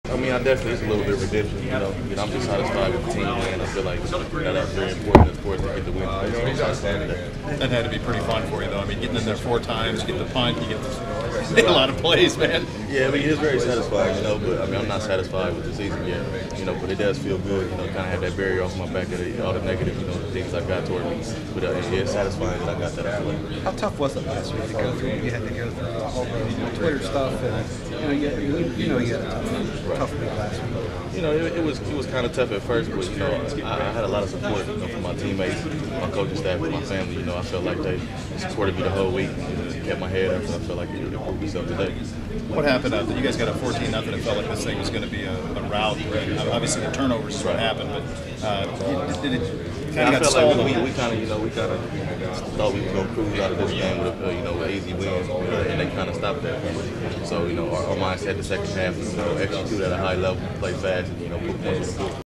Ohio State Fr. WR Jalin Marshall 11/22/2014
Buckeyes WR Jalin Marshall after OSU's 42-27 win over Indiana